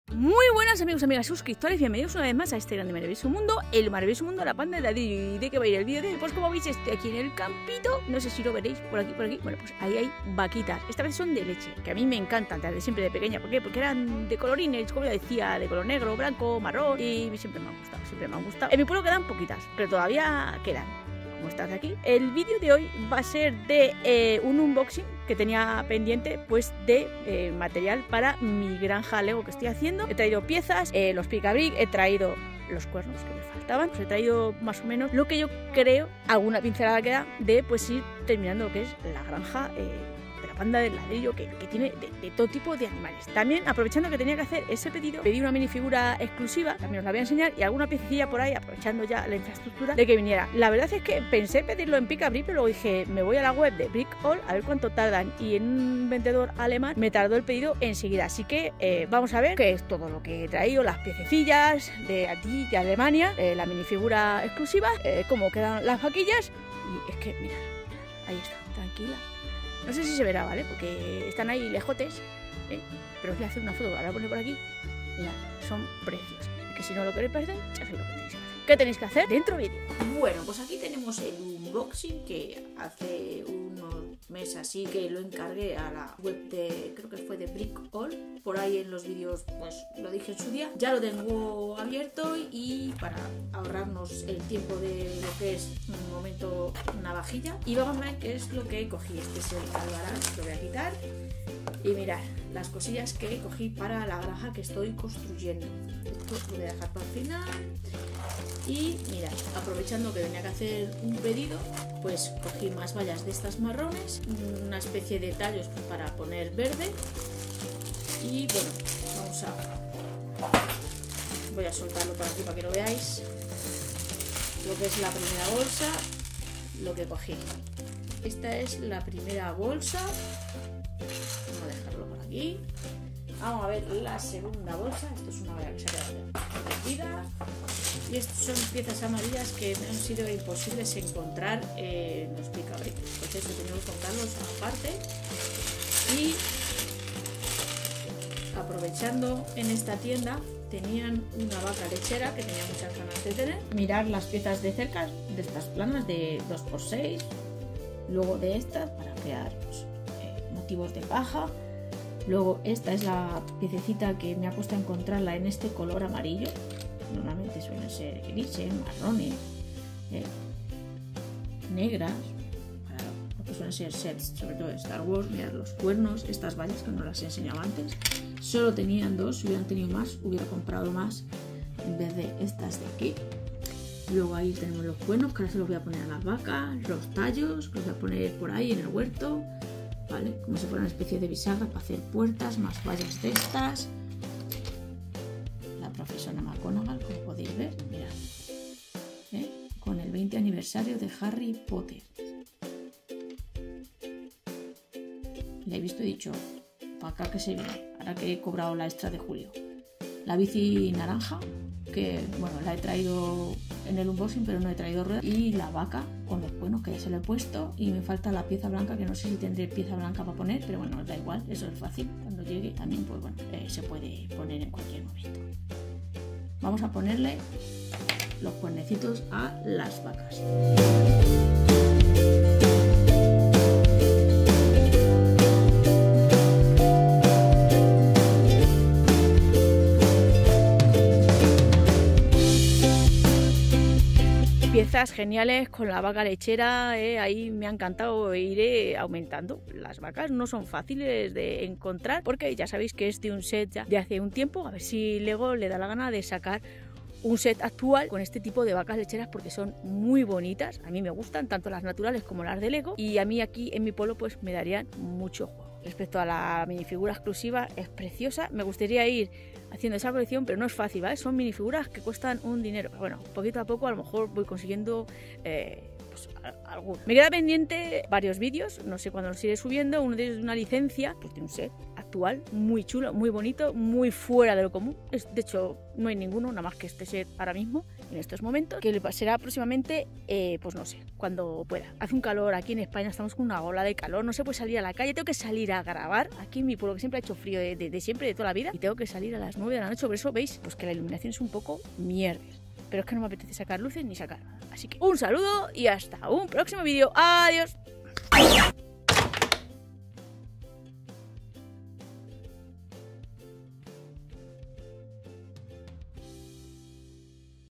Que NO se te olviden los cuernos
En este video te enseño umboxing de material lego que necesito para mi granja,en especial los cuernos de las vacas que se olvido comprar,hace unos meses
✅MIS ULTIMOS VIDEOS